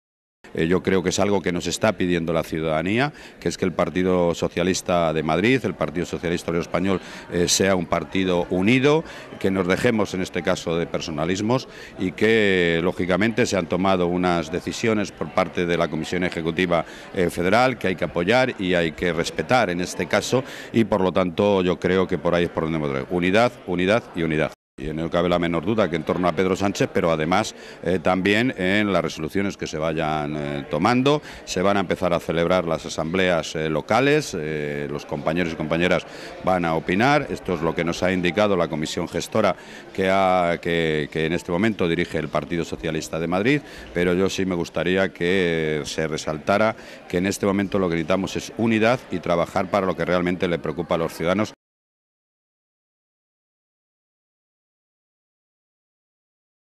Manuel Robles pide unidad en el PSM y dejar a un lado personalismos
Preguntado por las dos candidaturas presentadas en el Partido Socialista de Madrid para unas posibles primarias, Robles hace un llamamiento “a la unidad de toda la militancia socialista para trabajar en conseguir el éxito electoral” y solucionar los problemas de los ciudadanos. Unidad en torno al Secretario General, Pedro Sánchez y en “las resoluciones que se vayan tomando en las Agrupaciones locales” que empiezan este miércoles a reunirse.